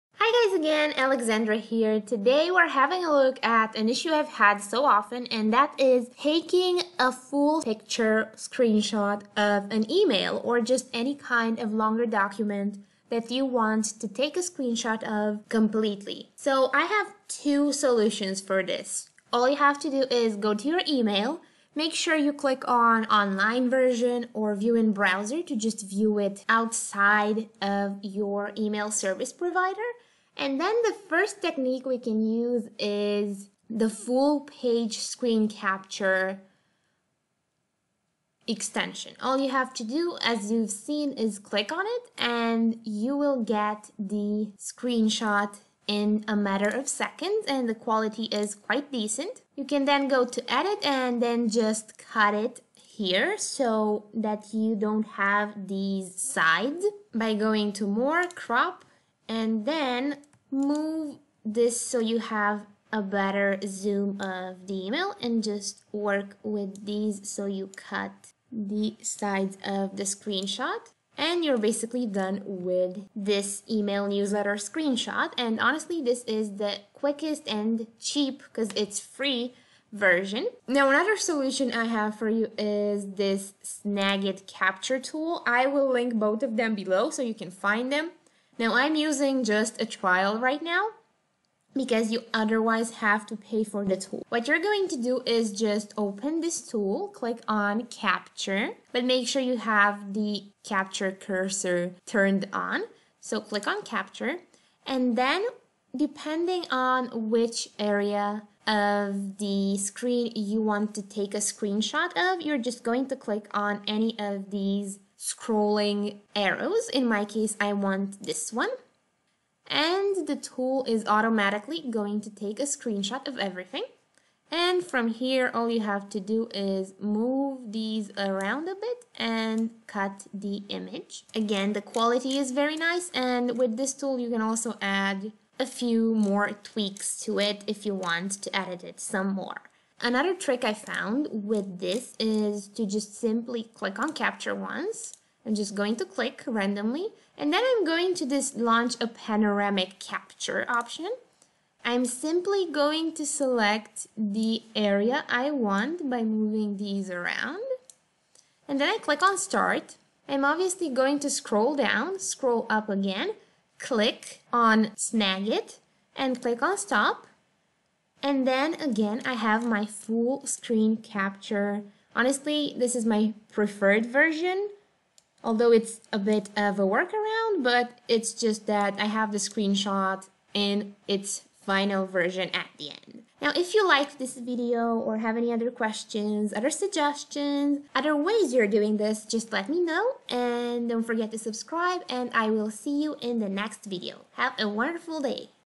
How to take a full page screenshot for emails - 2026 Tutorial for two different tools
How to take full screenshots of your emails. Full screen. Free option included.

Hope this tutorial helped!